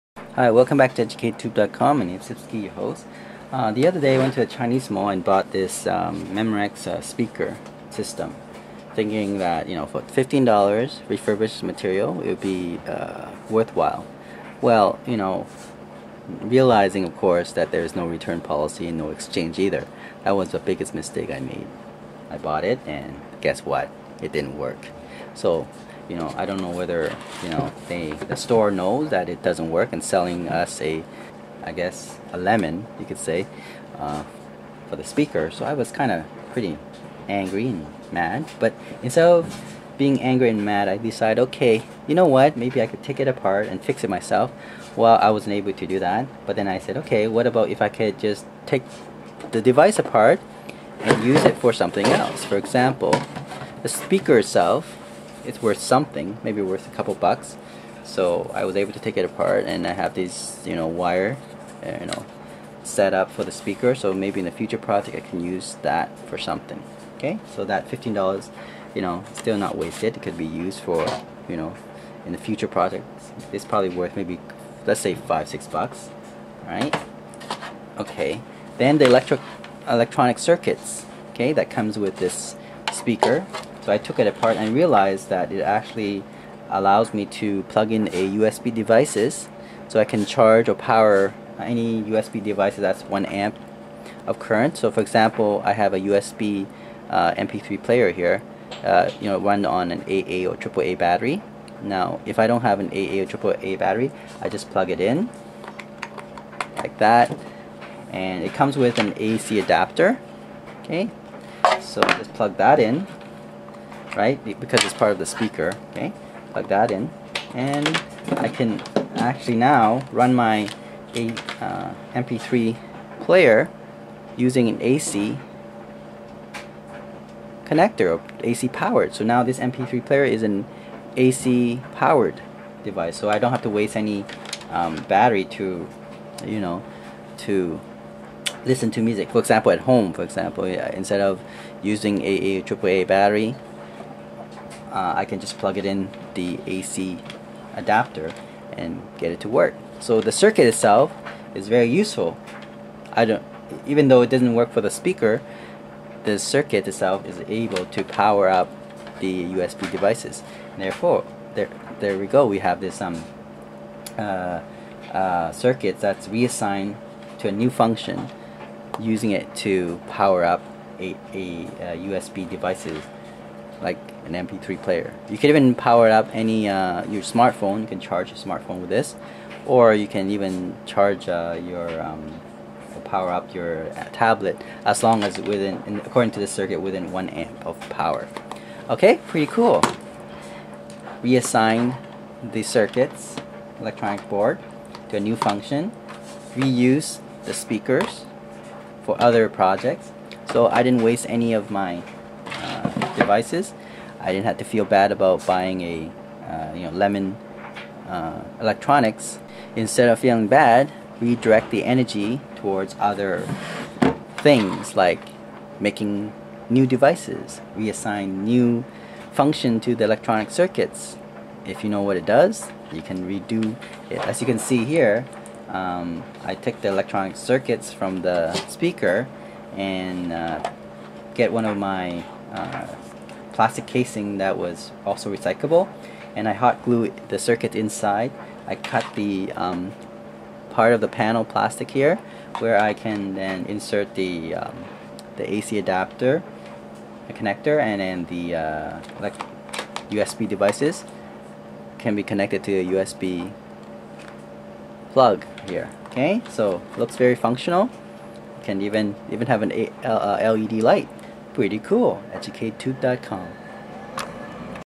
DIY AC adapter charger for MP3 player from broken speaker
Salvage broken speaker into useable parts.  The electronic component is reassigned to become an USB charger or power for MP3 player, tablet or smartphone chargers.